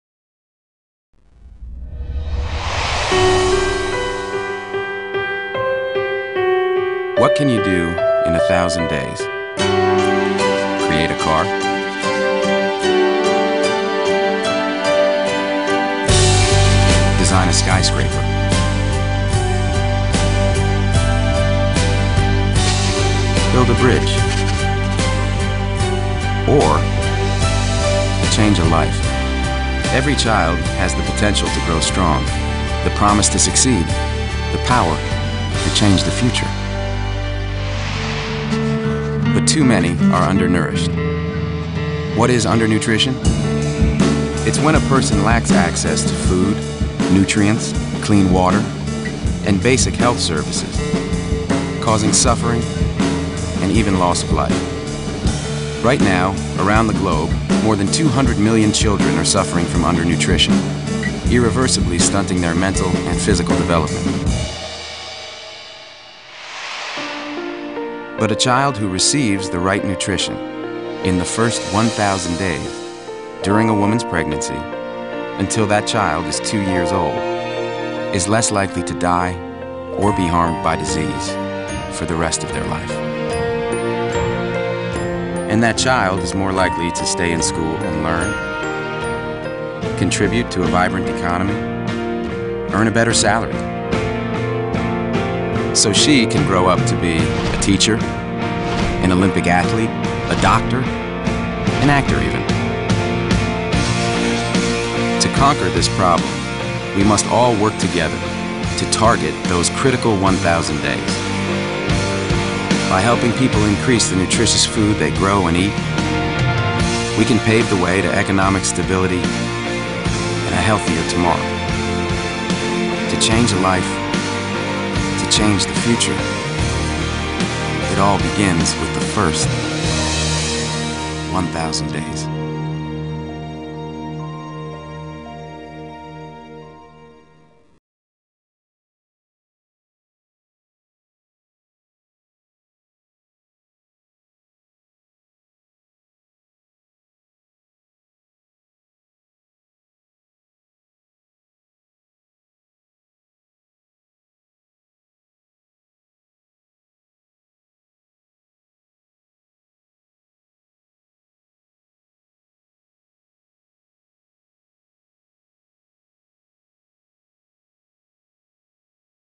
What can you do in 1,000 Days?
First 1000 days explains the beginning of baby's birth and growth from the mother's womb up to 24 months of life. This issue was treated during the inauguration of the first breastfeeding room in Cameroon on the 16th June 2017. Companies are invited to copy Nestle's example. Nestle has 400 breastfeeding rooms in enterprise arund the world.